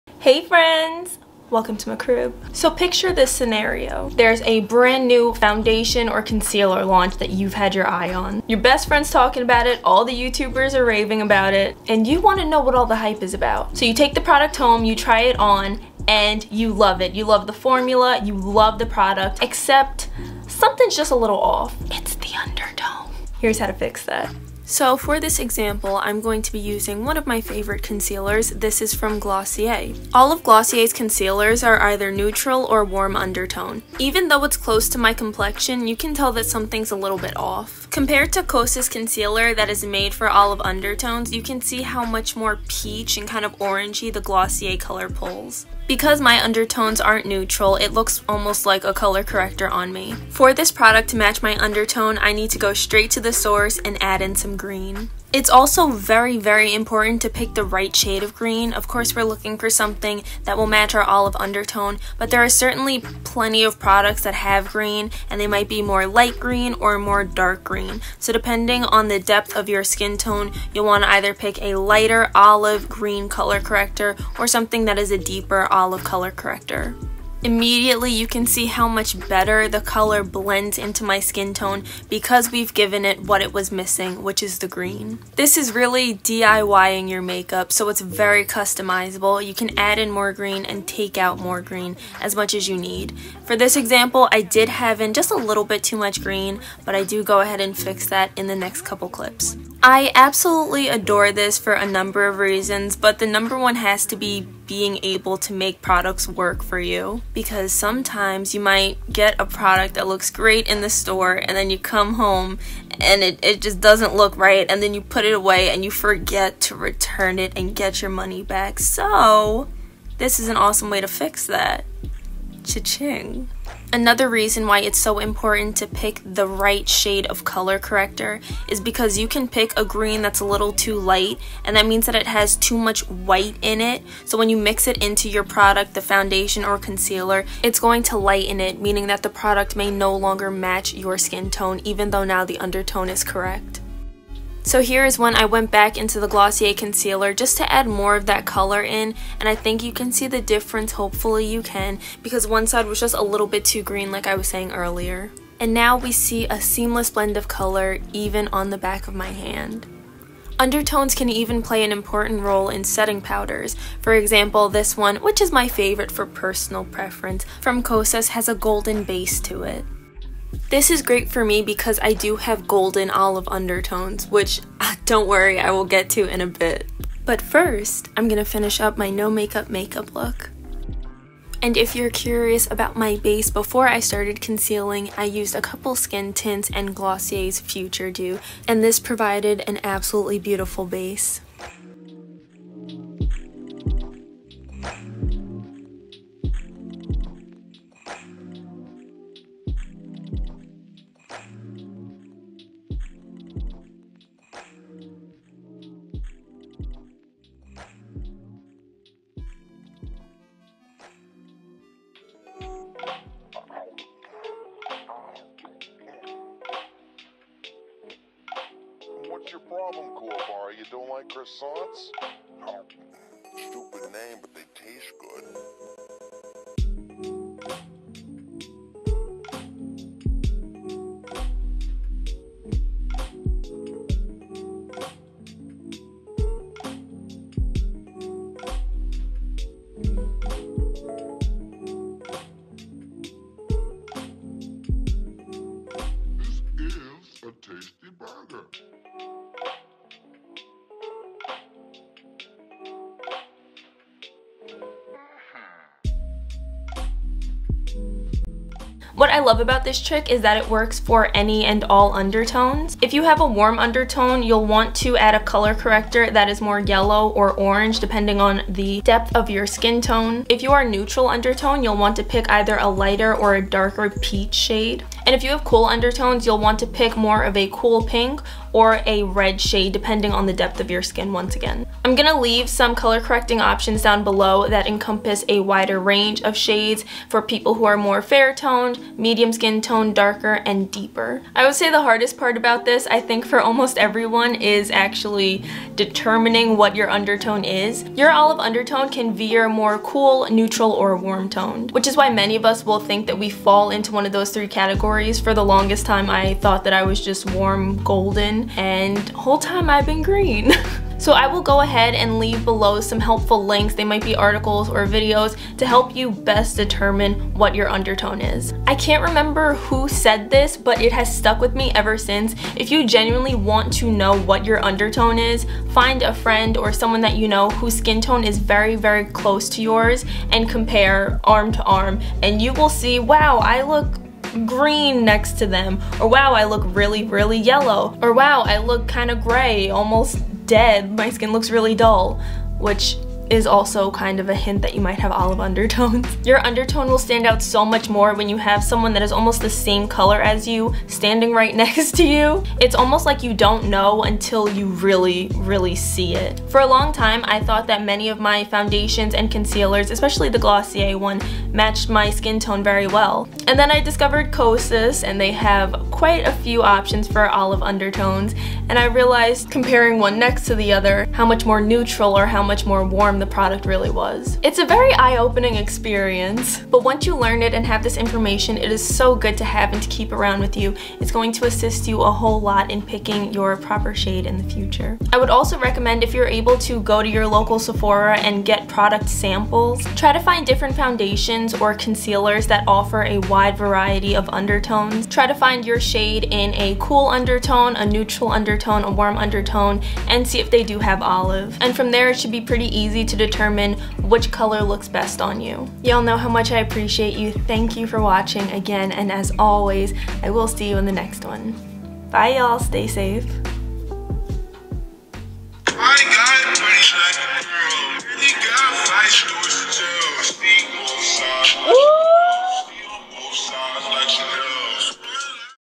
my frens with olive undertones, this one’s to you…make any foundation match YOU neutral, cool & warm
✨Need some one on one skincare help? Book a virtual skin consultation with me!

Here's my favorite way to fix any foundation, concealer, cc cream, skin tint, all complexion stuff so it ACTUALLY matches olive undertones.

la girl pro green corrector concealer

other products used:
~my base makeup products~
glossier futuredew
kosas skin tint 5.5 + 6
glossier perfecting skin tint G6

glossier stretch concealer G6
kosas revealer concealer tone 6
kosas cloud set in cushiony
benefit hoola bronzer
black radiance baked blush in toasted almond
freck og freckle pen
bite beauty intensive lip treatment mask

~~IMPORTANT TIP: I understand it can be harder for deeper skin tones especially to find color correctors that are dark enough. Try mixing a darker matte eyeshadow with some concealer to create your own perfect shade of color corrector! This hack will work with any color you want to use as a corrector :) Check out some of my recommendations below! ~~

COOL UNDERTONES (pink/red corrector)
fair/light/medium
1. la girl pro corrector concealer in cool pink
2. nars liquid color corrector in light
3. ulta color correcting liquid concealer in pink

tan/dark/deep
1. la girl pro corrector concealer in red
2. j.cat beauty quad color corrector
3. nars liquid color corrector in deep & medium deep

NEUTRAL UNDERTONES (peach corrector)
fair/light/medium
1. la girl pro corrector concealer in peach
2. flower beauty color corrector in light peach & medium peach
3. nars liquid color corrector in medium

tan/dark/deep
1. flower beauty color corrector in medium peach & deep peach
2. black radiance hd corrector concealer in peach & salmon
3. nyx dark circle concealer in deep & medium

WARM UNDERTONES (yellow/orange corrector)
fair/light/medium
1. la girl pro corrector concealer in light yellow & yellow
2. j.cat beauty quad color corrector
3. lottie london colour correcting palette

tan/dark/deep
1. la girl pro corrector concealer in yellow & orange
2. dragun color corrector in orange
3. black radiance hd corrector concealer in orange & yellow

OLIVE UNDERTONES (green corrector)
fair/light/medium
1. la girl pro corrector concealer in green
2. erborian cc red correct
3.  lottie london colour correcting palette

tan/dark/deep
1. j.cat beauty quad color corrector
2.  la girl pro corrector concealer in green (*may lighten original foundation color)
3. elf color correcting stick

background music (slowed versions)